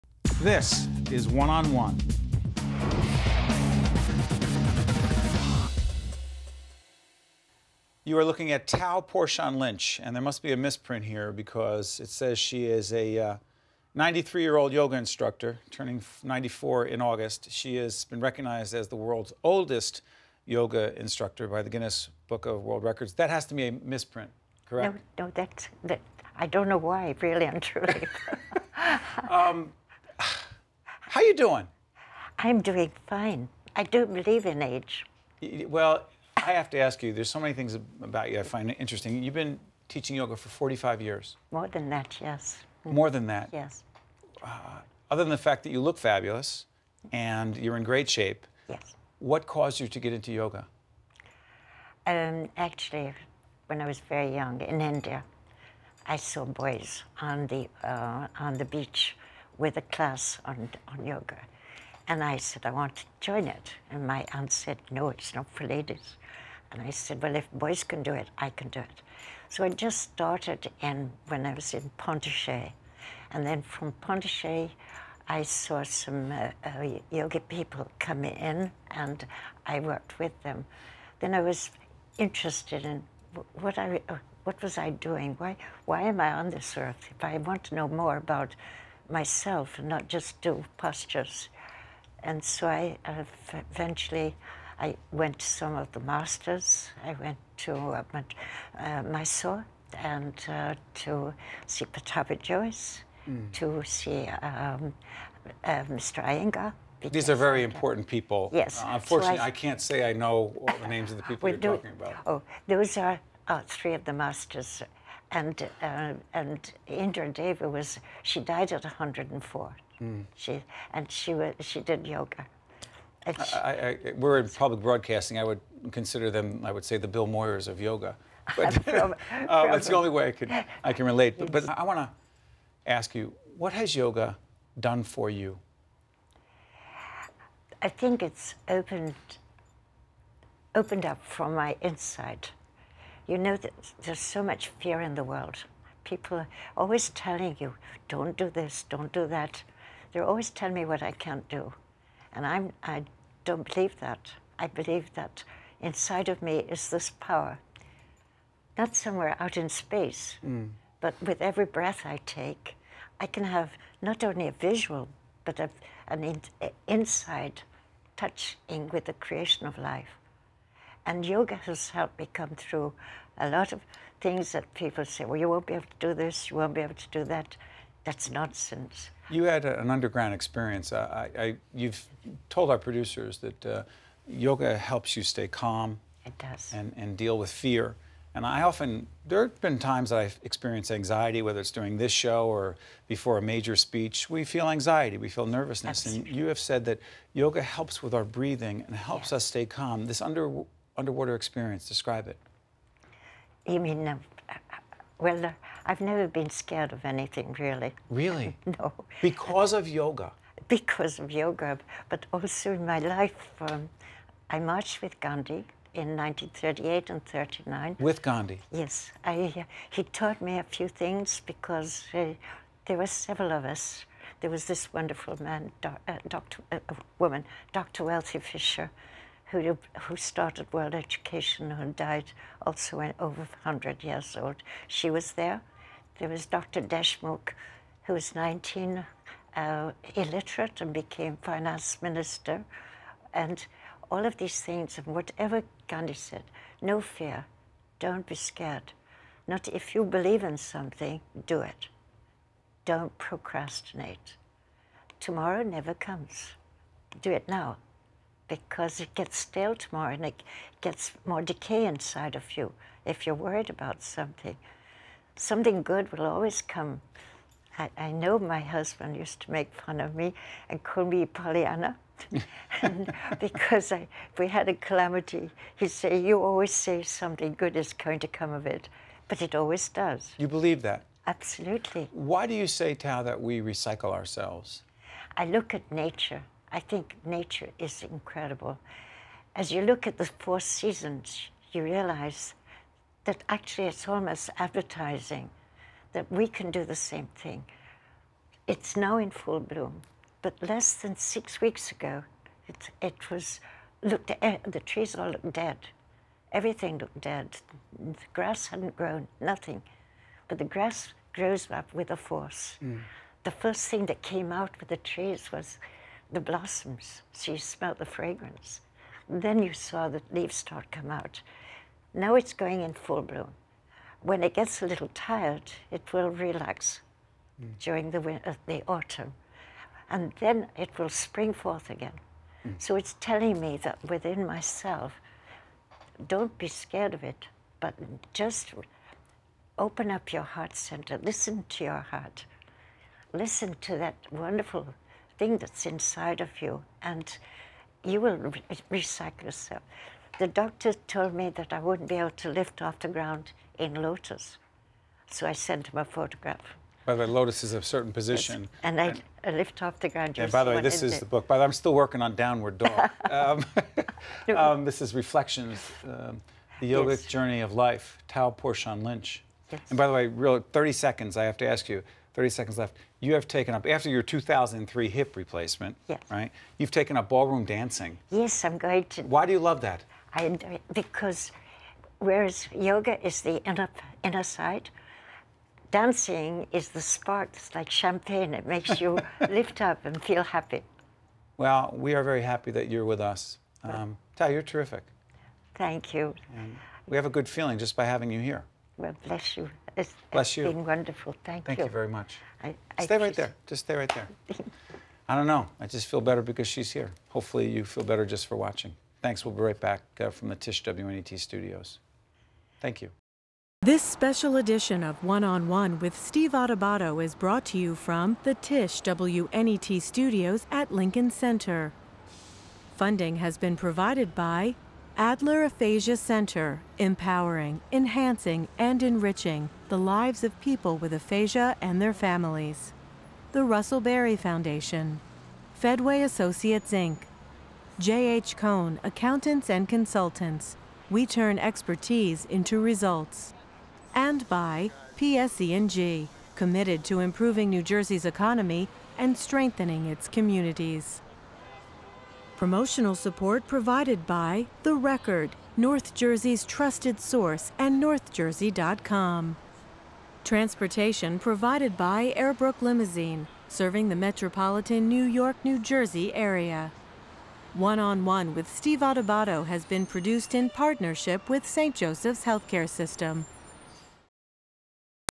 Benefits of Yoga From 93 Year Old
93-year-old yoga instructor, Tao Porchon-Lynch, explains the benefits of yoga and her positive outlook on life and age.